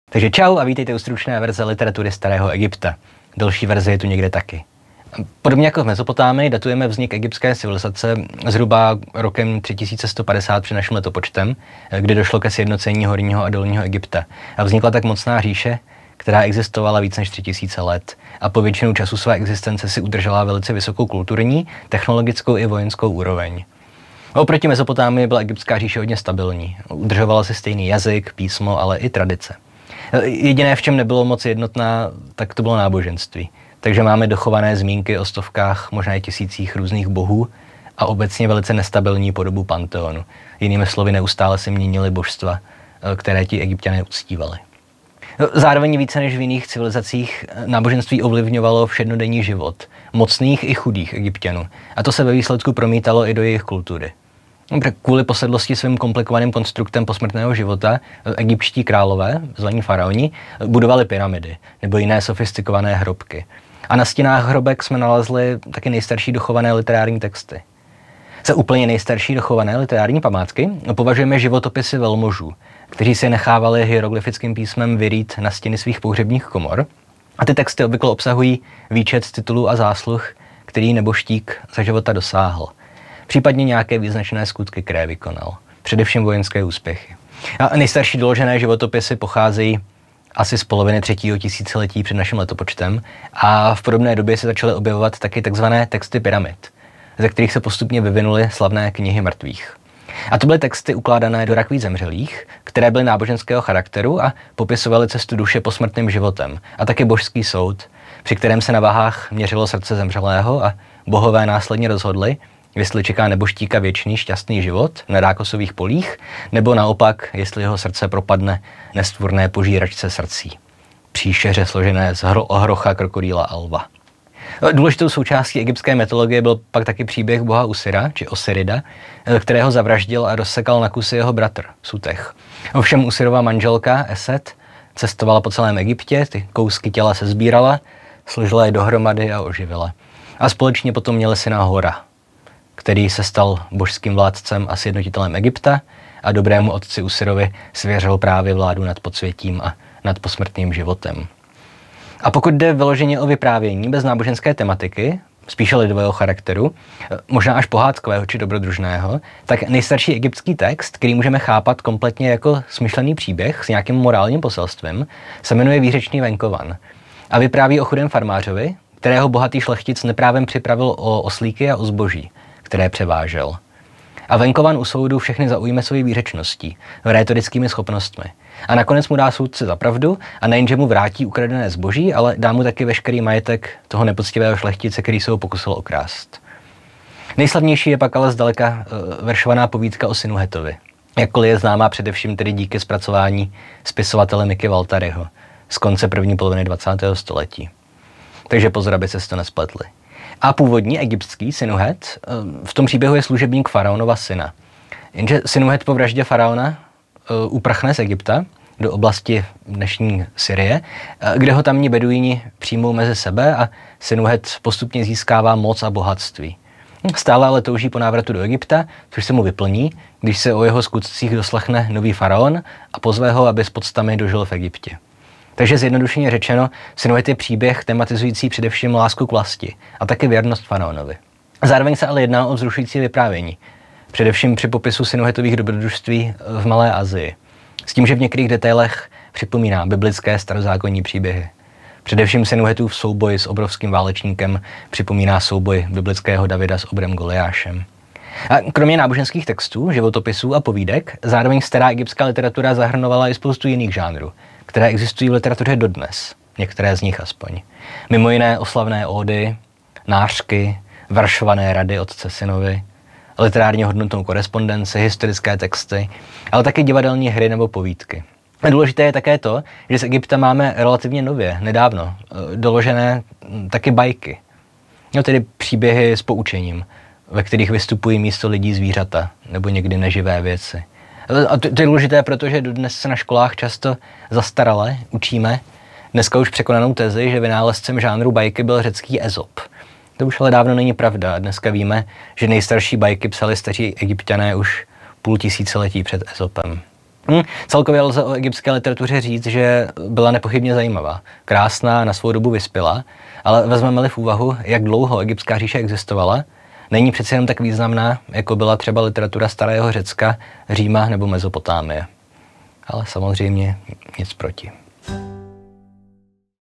PPL2 - literatura starého egypta - krátká verze
Čau! Dnes se podíváme na literaturu starého Egypta a bude to prima bžunda.

Chcete naši tvorbu podpořit? Za €5 měsíčně můžete mít přístup do Discoru přes naše Herohero

Moc děkujeme všem našim podporovatelům!
David Jirsa & Daniel Zezula